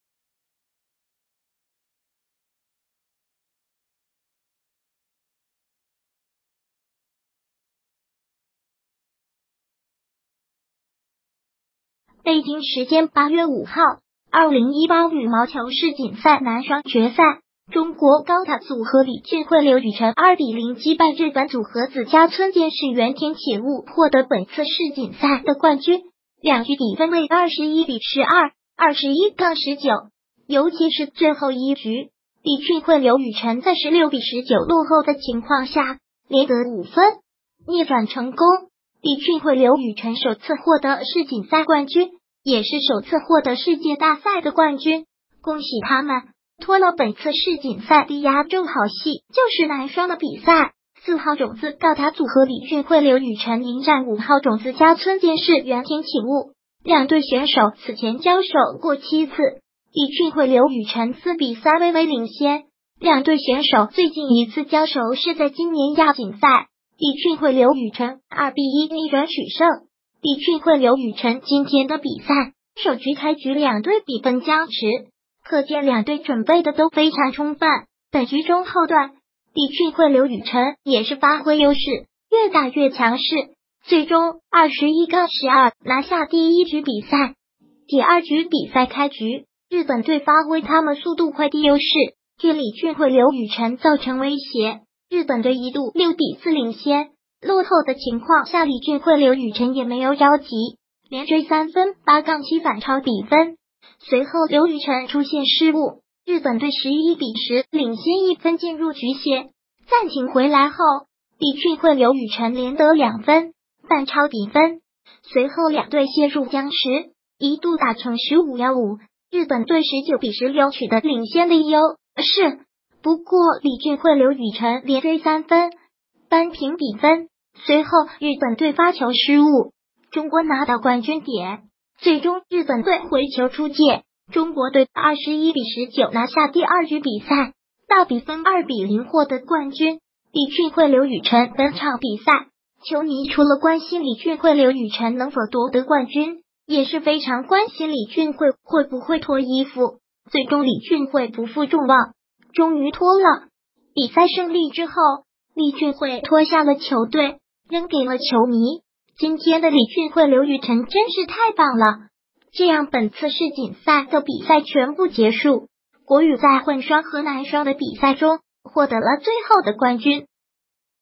脱了，李俊慧/刘雨辰末局连得5分，2-0击败日本队获得男双冠军！
北京时间8月5号，2018羽毛球世锦赛男双决赛，中国“高塔组合”李俊慧/刘雨辰2-0击败日本组合子嘉村健士/园田启悟获得本次世锦赛的冠军，两局比分为21-12，21-19。尤其是最后一局，李俊慧/刘雨辰在16-19落后的情况下，连得5分，逆转成功。李俊慧/刘雨辰首次获得世锦赛冠军，也是首次获得世界大赛的冠军，恭喜他们！脱了本次世锦赛的压轴好戏就是男双的比赛，4号种子“高塔组合”李俊慧/刘雨辰迎战5号种子嘉村健士/园田启悟。两对选手此前交手过7次，李俊慧/刘雨辰4-3微微领先。两对选手最近一次交手是在今年亚锦赛，李俊慧/刘雨辰2-1逆转取胜。 李俊慧/刘雨辰今天的比赛，首局开局两队比分僵持，可见两队准备的都非常充分，本局中后段，李俊慧/刘雨辰也是发挥优势，越大越强势，最终21-12拿下第一局比赛。第二局比赛开局，日本队发挥他们速度快的优势，给李俊慧/刘雨辰造成威胁，日本队一度6-4领先。落后的情况下李俊慧/刘雨辰也没有着急，连追3分8-7反超比分。随后刘雨辰出现失误，日本队11-10领先1分进入局歇。暂停回来后，李俊慧/刘雨辰连得2分，反超比分。随后两队陷入僵持，一度打成15-15.日本